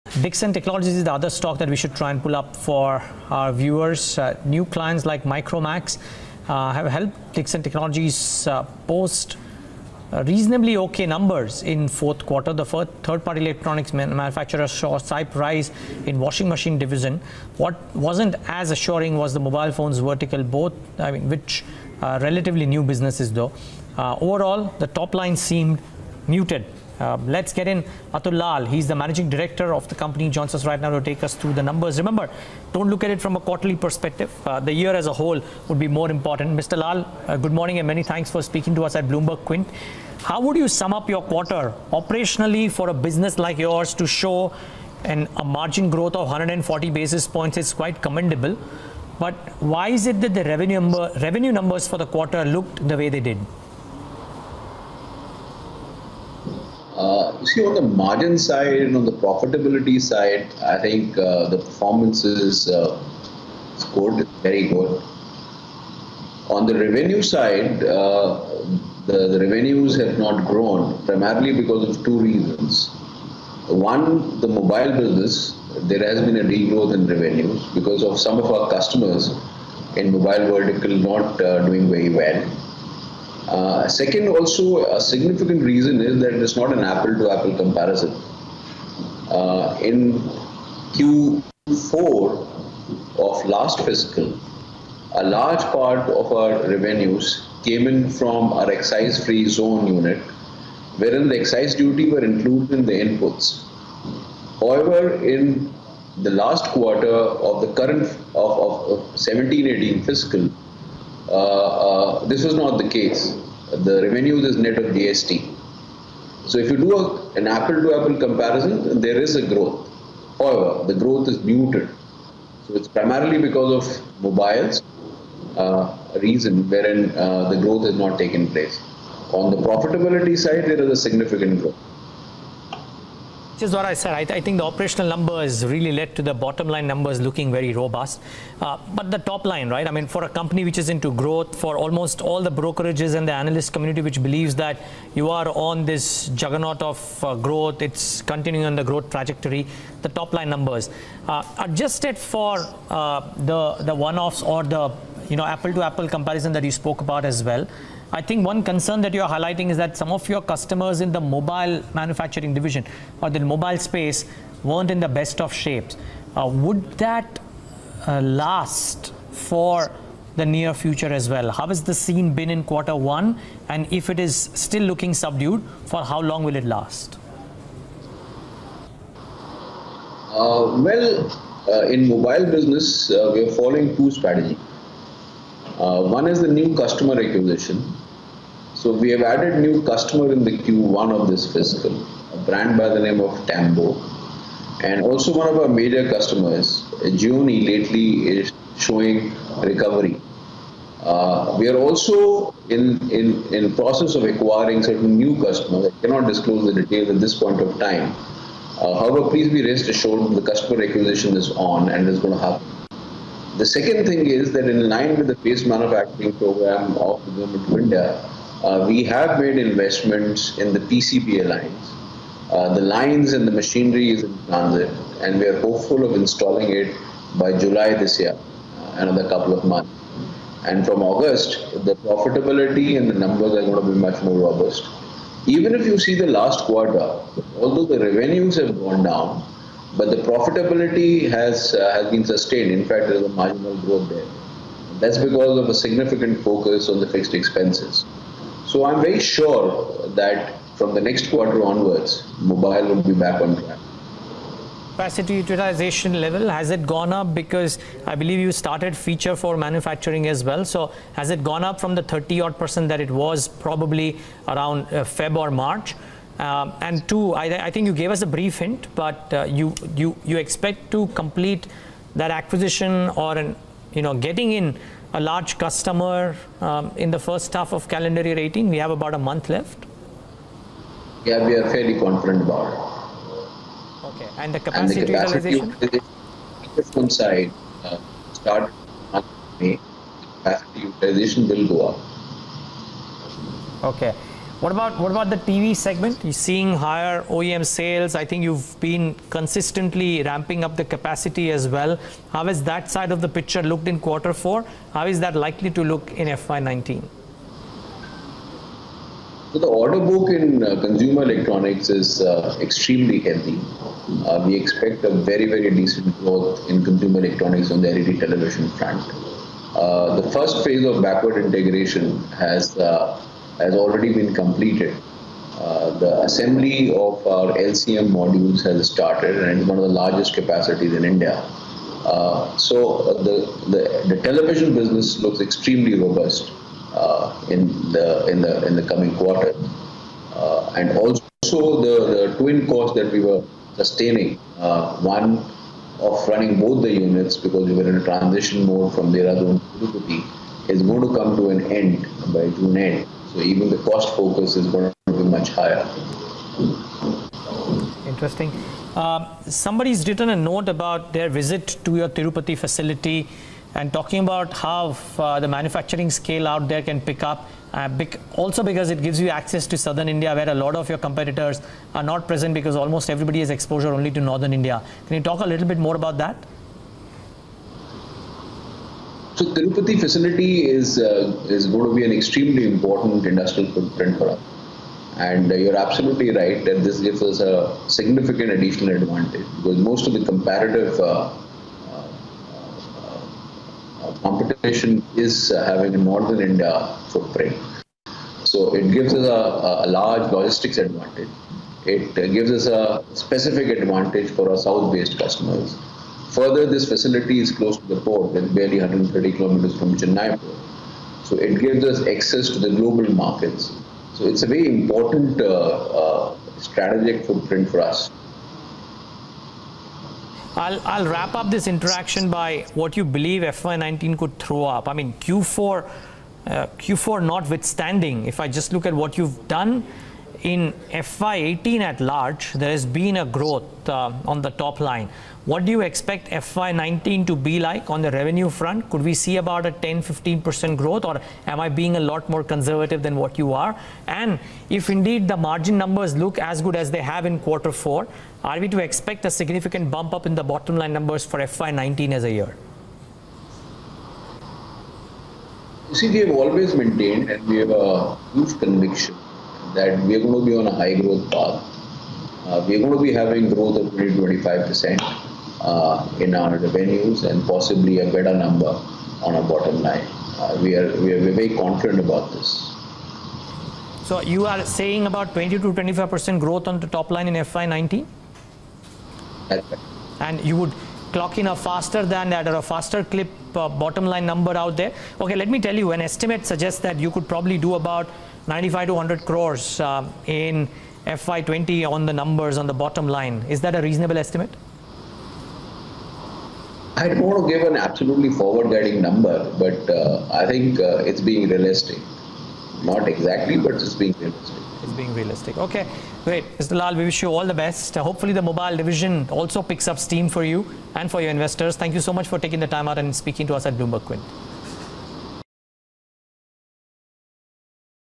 Dixon Technologies: Expect 20-25% Topline Growth In FY19
Q4 With BQ: Decoding Dixon Technologies' earnings with MD Atul Lall.